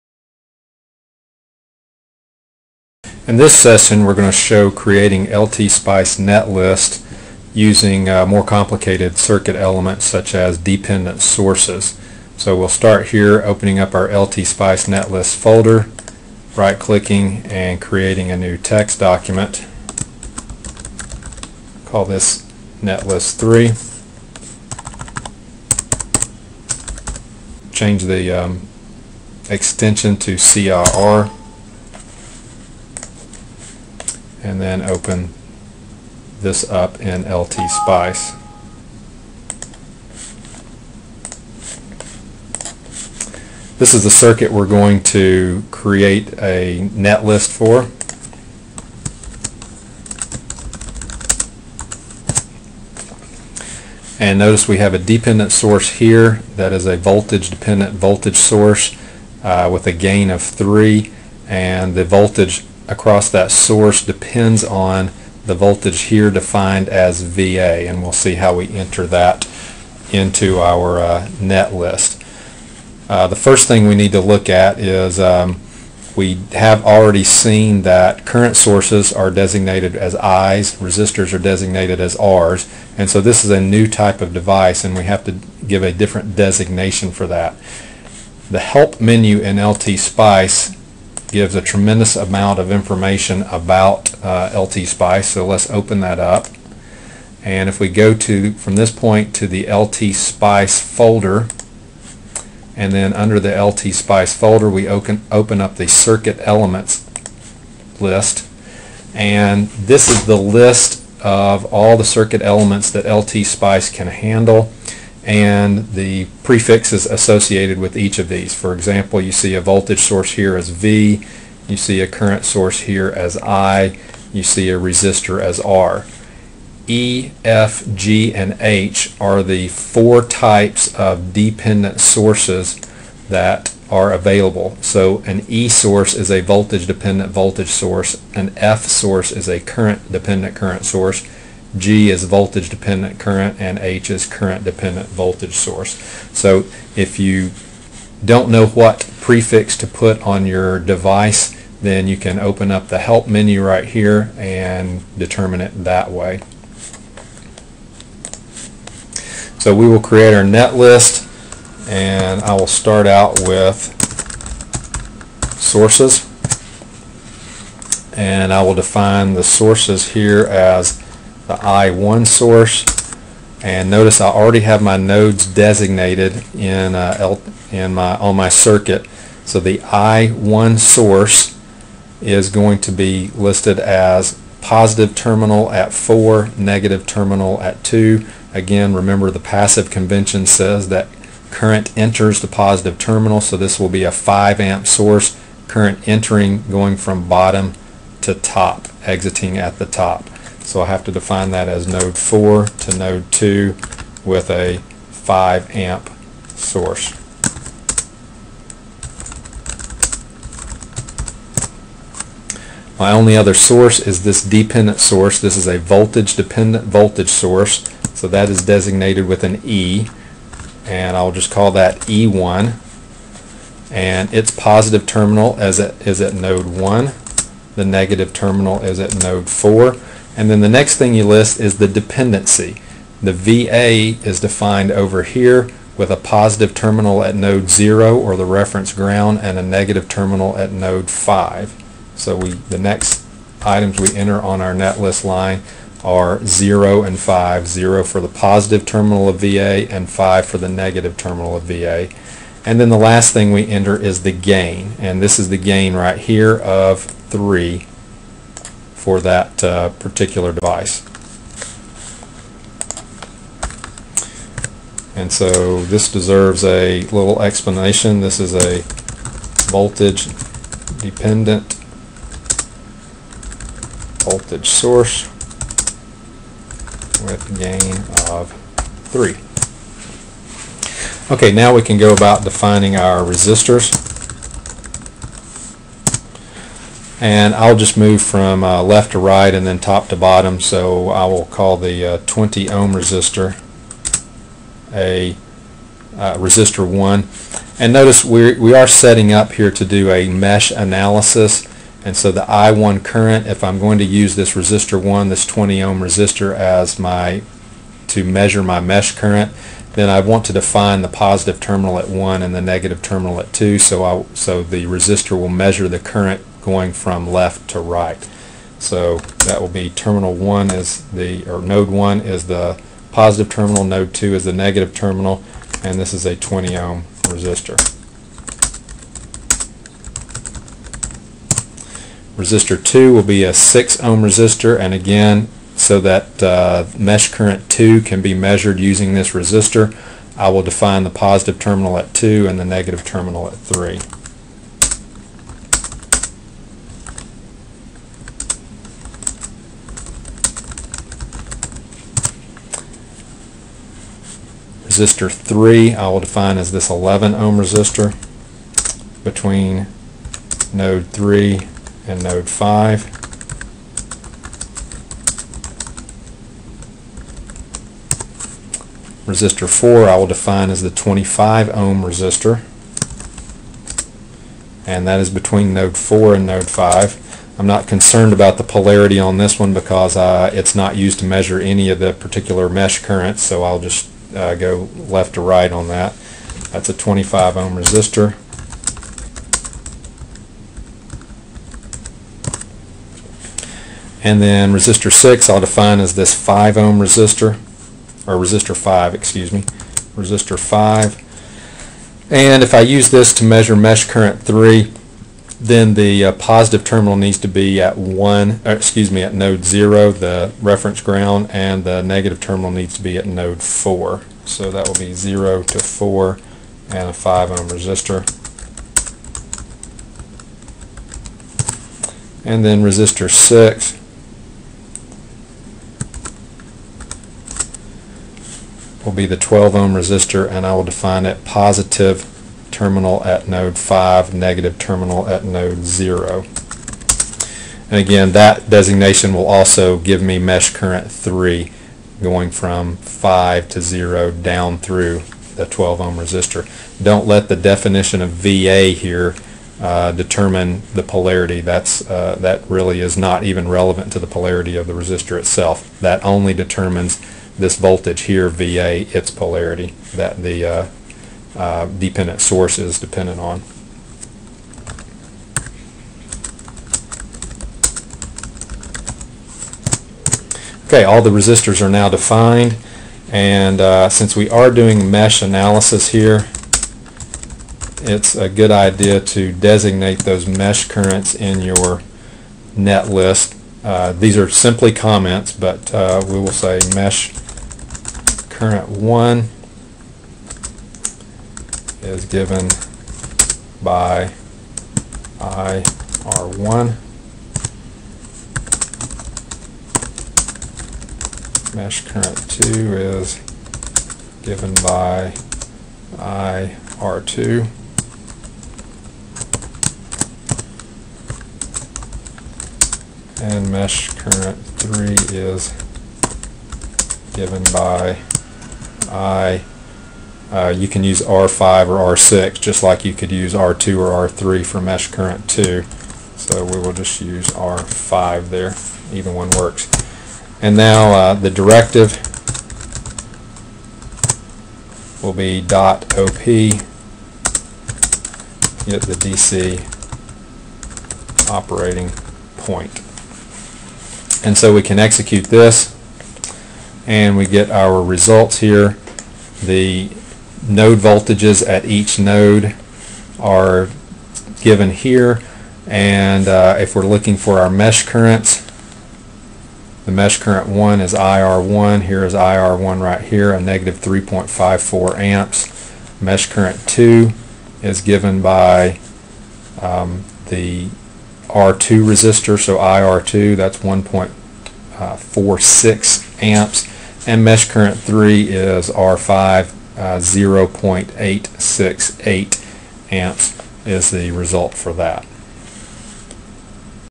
LT Spice Netlists tutorial 3
Part 3 of 4. Tutorial on creating netlists in LTSpice. This tutorial covers voltage dependent voltage sources.